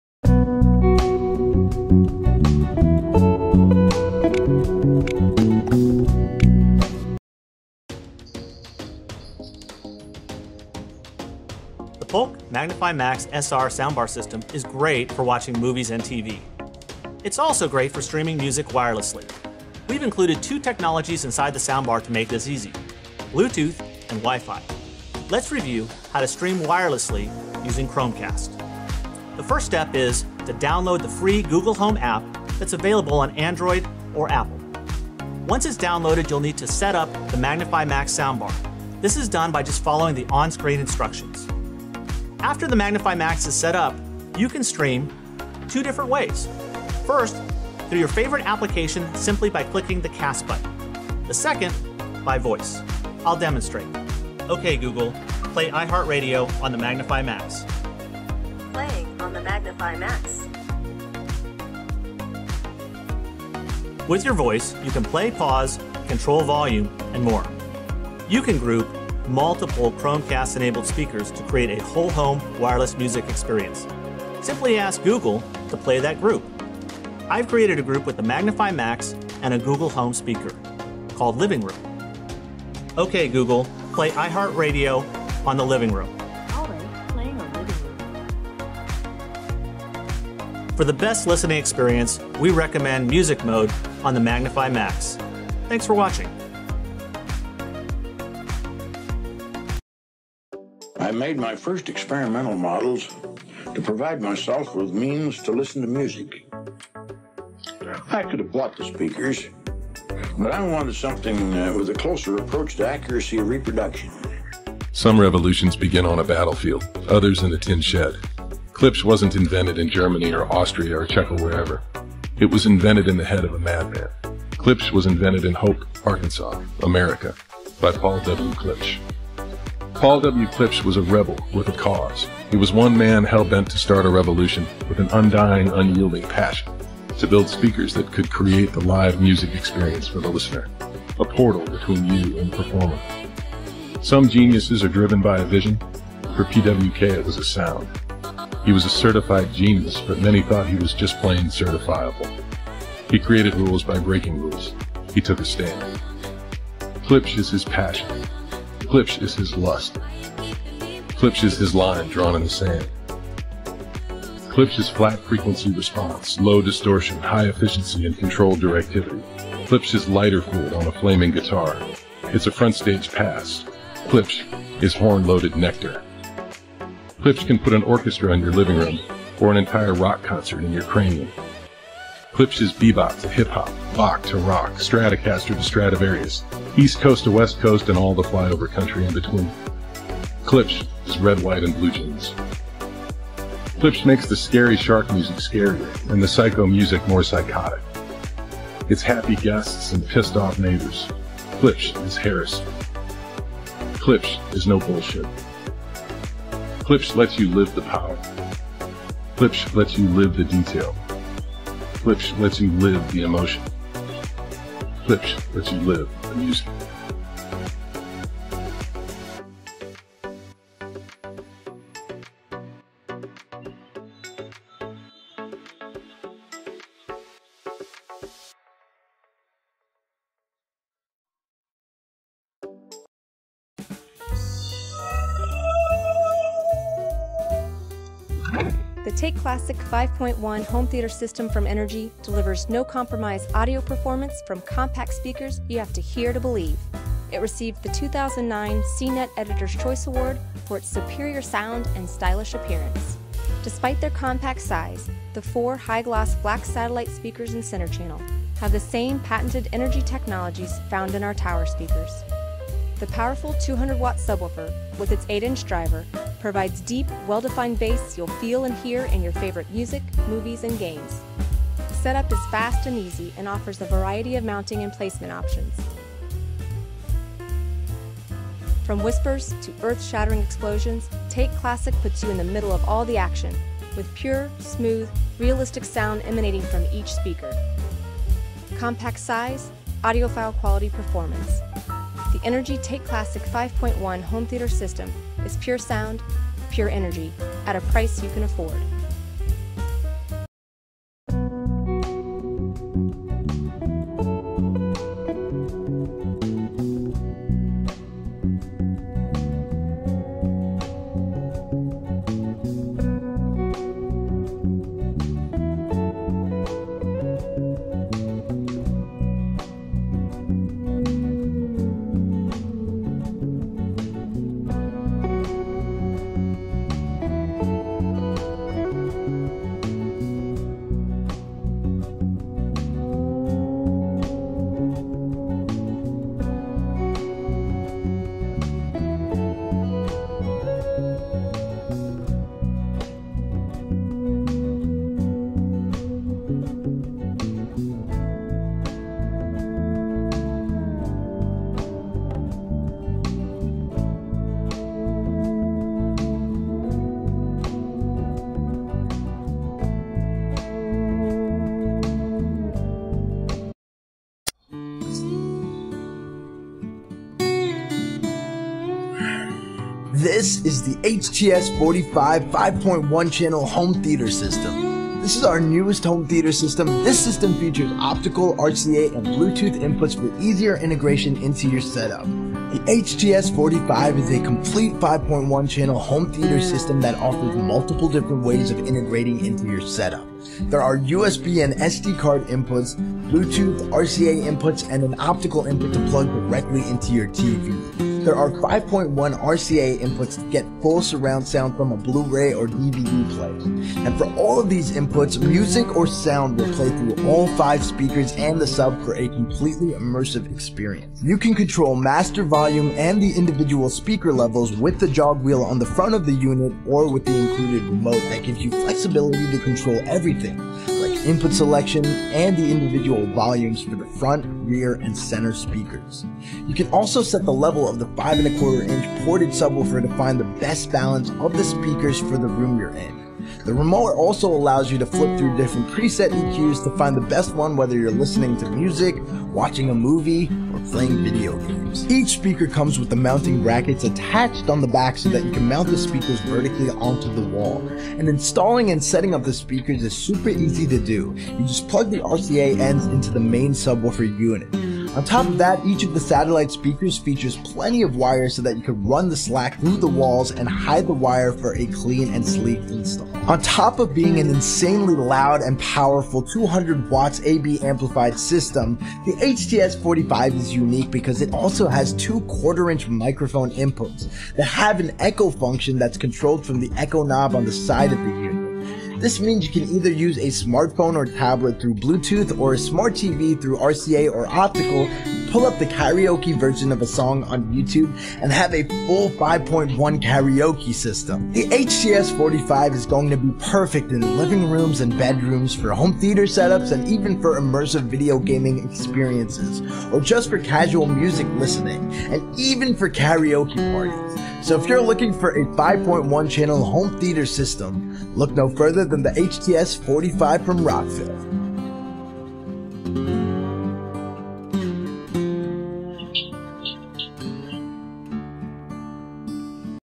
Best Home Theatre System Surround Sound With Receiver 2022 ✅
Why should you invest in Home Theatre System? If you want to buy a home theater system that makes your visual experience thoroughly thrilling, you need to consider all the details. There is no point spending a large amount of money on a product without going through its features in full. There is a real thrill in having a classy home theater system that meets your needs and adds value to your residence. We present to you: Best Home Theatre System Surround Sound With Receiver 2022.

👉1. Polk Audio MagniFi
👉2. Klipsch HDT-600
👉3. Energy 5.1 Take Classic
👉4. Bobtot Home Theater
👉5. Rockville HTS45

Check Now: Best Home Theatre System Surround Sound With Receiver 2022

No company has paid to be a part of this list. We received no sponsorship money in the creation of this video.

This video is a brief presentation of the Best Home Theatre System Surround Sound With Receiver 2022.